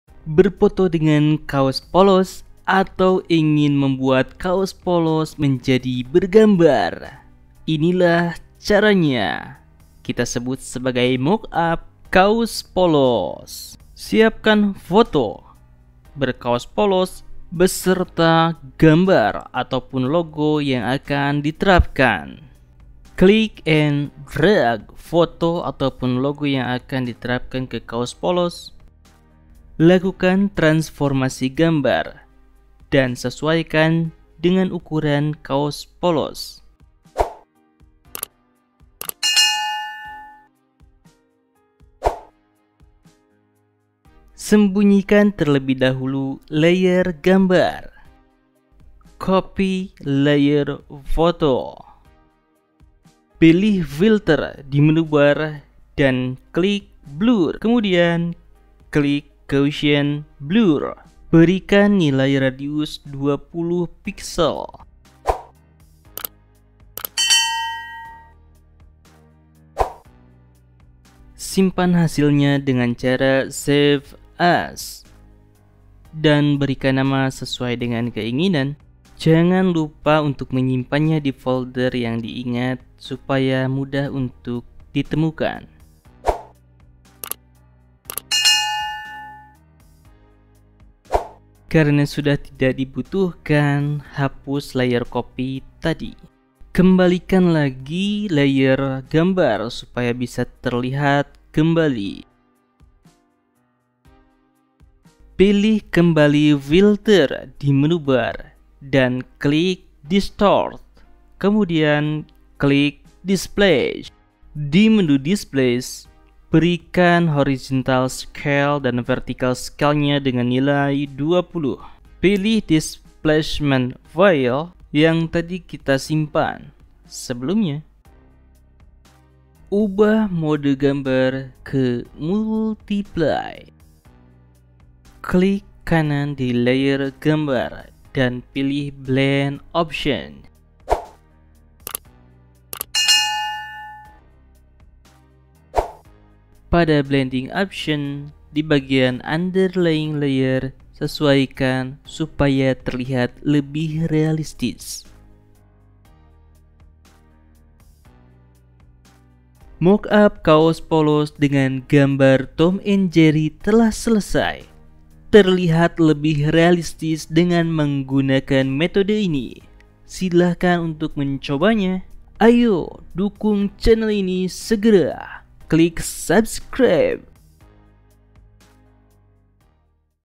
Pasang Logo di Kaos Menggunakan | Tutorial Mockup Photoshop FULL
Dalam video ini menunjukan cara Pasang Logo di Kaos Menggunakan Photoshop  termasuk kedalam kategori Tutorial Mockup gak pake lama,  mudah untuk di edit dengan semua detail yang diberikan , menggunakan teknik zoom in dan zoom out sehingga akan mudah dipahami, dengan bantuan edit menggunakan software adobe photoshop semuanya menjadi terasa lebih mudah untuk belajar berbagai desain grafis. Mari belajar mulai dari SEKARANG !!!
Ikuti Juga Saya di :
Software yang Digunakan : Photoshop cc 2022